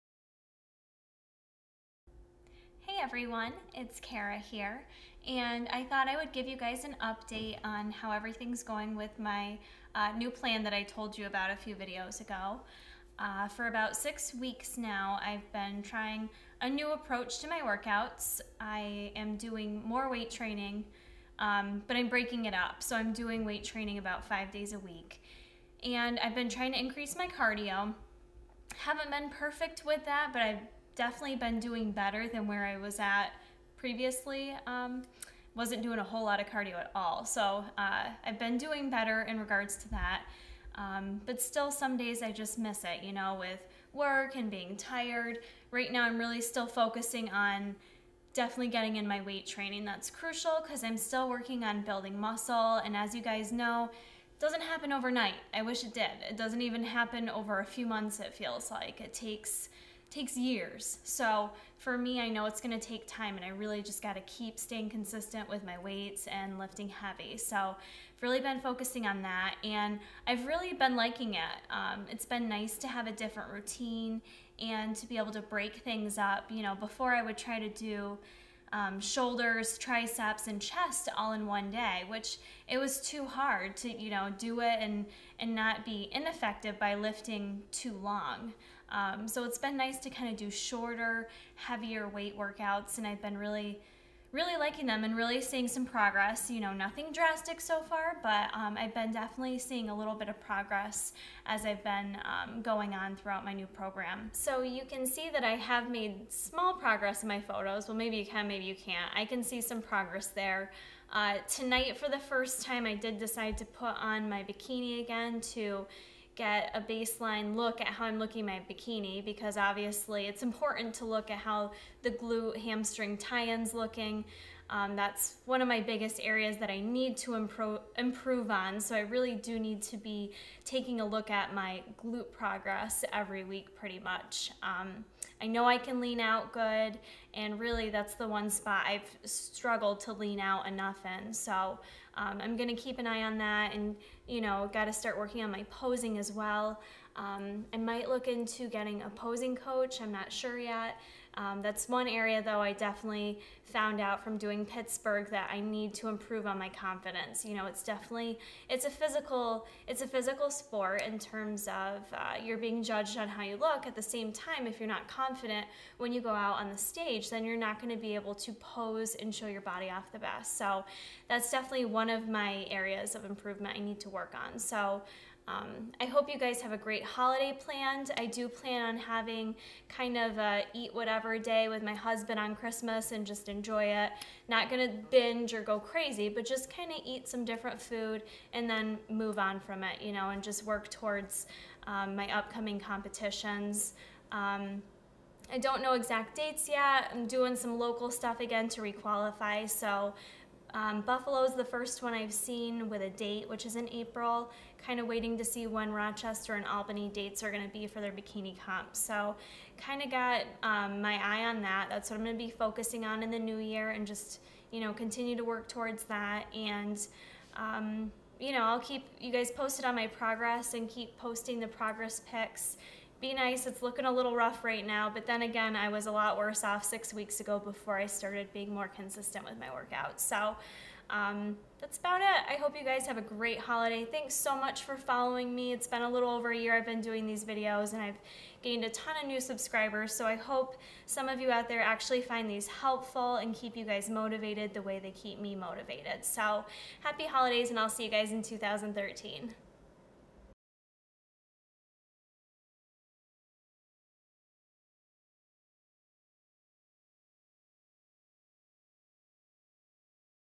Bikini Training Update
Just wanted to update everyone on my training for the past 6 weeks and the progress I have made.